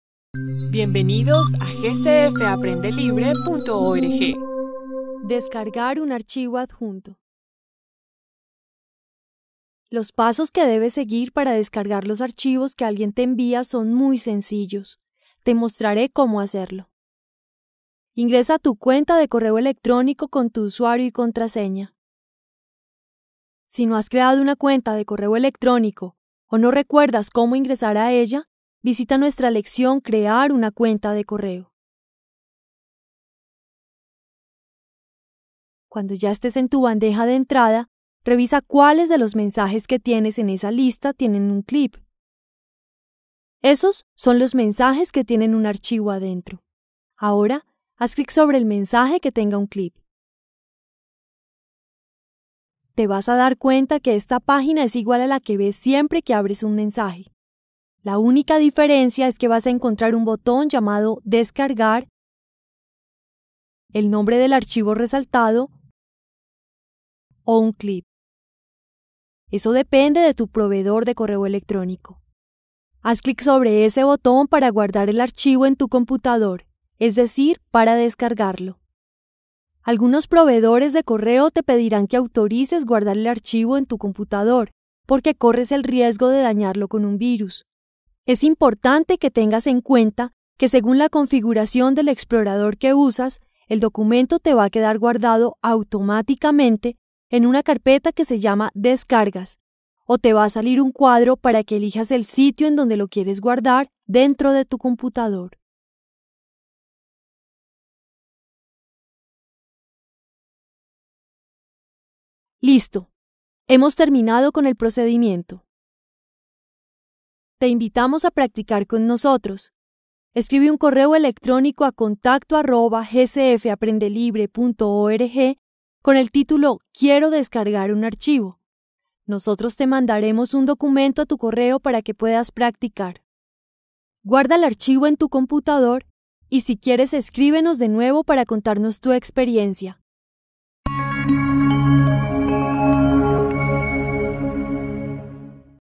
Cómo descargar un archivo desde Hotmail | CÓMO CREAR UN CORREO ELECTRÓNICO EN HOTMAIL
En esta clase del curso "Cómo crear un correo electrónico en Hotmail" aprenderás cómo descargar un archivo desde Hotmail, cómo descargar un archivo adjunto en Hotmail, cómo descargar un documento desde Hotmail, cómo saber si un correo tiene un archivo adjunto en Hotmail, dónde queda guardado un archivo cuando lo descargo.

Si ingresas a la bandeja de entrada de tu correo electrónico y ves un mensaje con un clip, significa que este contiene un archivo adjunto. Aprende cómo descargar estos archivos en tu computador.